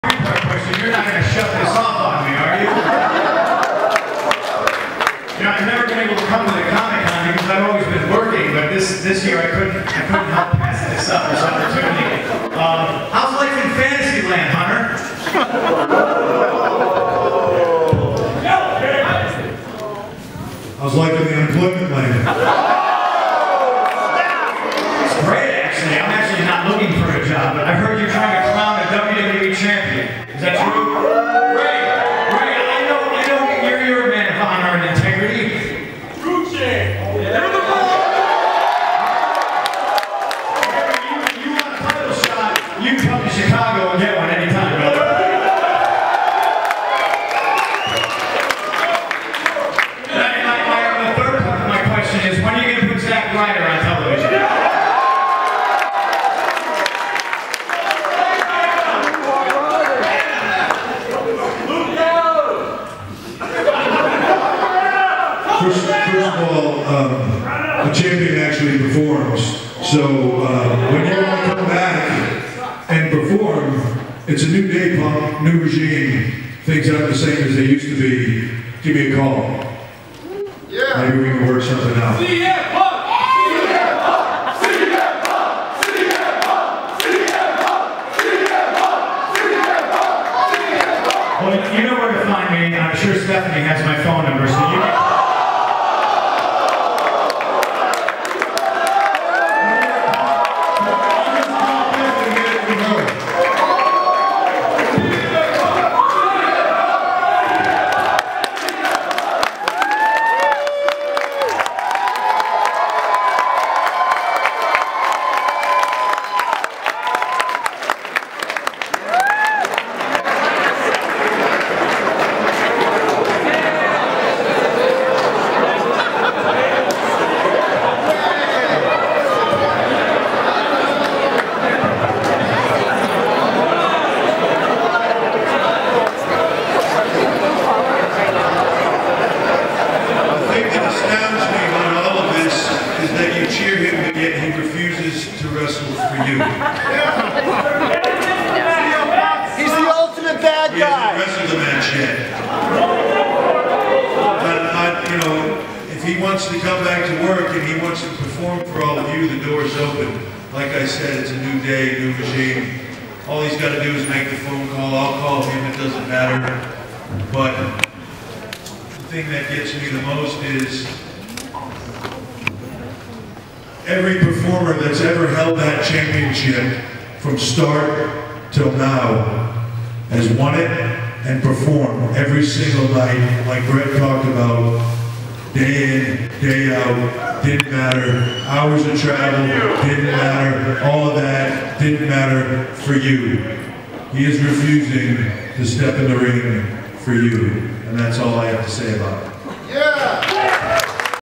CM Punk crashes Comic-Con 2011 - Another angle
CM Punk crashing WWE's Mattell panel at San Diego Comic-Con 2011. Yet another angle. I shot this!